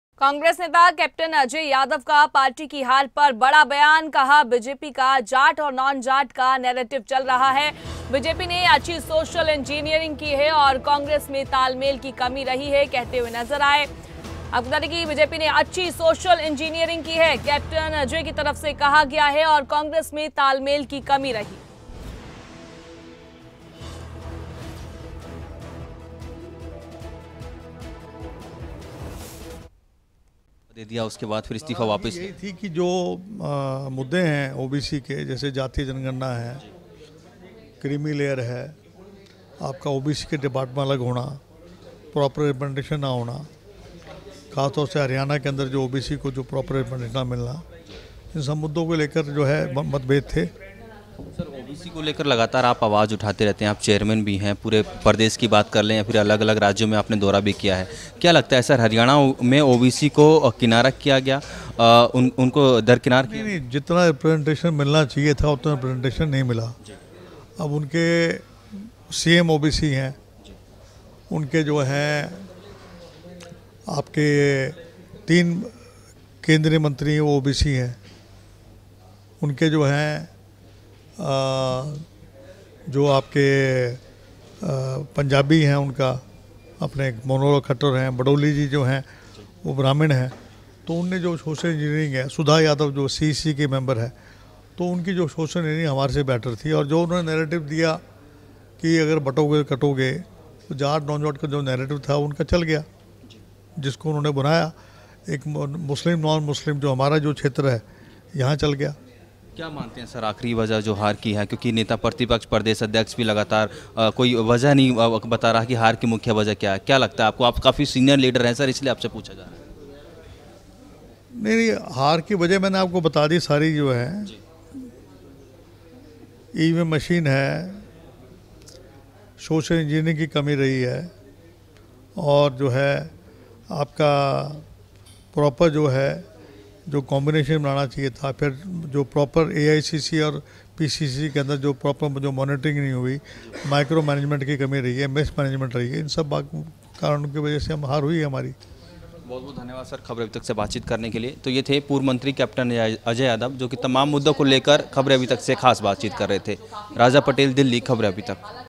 पूर्व मंत्री कै.अजय यादव से EXCLUSIVE बातचीत, इस्तीफा वापस लेने और कांग्रेस की हार पर दिया बड़ा बयान
कांग्रेस नेता कैप्टन अजय यादव से EXCLUSIVE बातचीत
कैप्टन अजय यादव का हरियाणा में कांग्रेस की हार पर बड़ा बयान
बीजेपी का जाट और नॉन जाट का नैरेटिव चल गया- अजय यादव
बीजेपी ने अच्छी सोशल इंजीनियरिंग की- अजय यादव
कांग्रेस में तालमेल की कमी रही- कै.अजय यादव